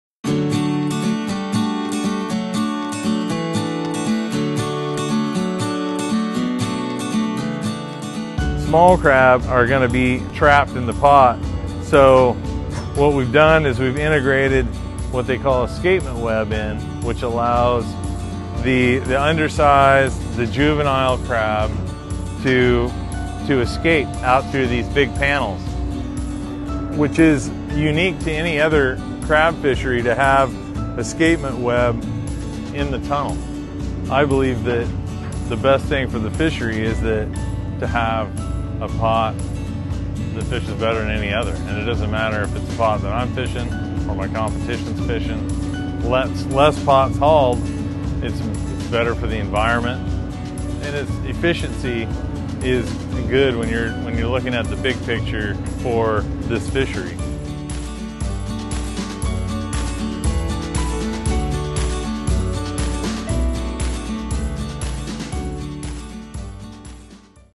Building a Better Pot - Golden King Crab Pots
Incorporating an escapement web into the tunnel of the Golden King crab pots allow undersized and juvenile crabs to escape, ensuring a more efficient catch and better protection for the fishery.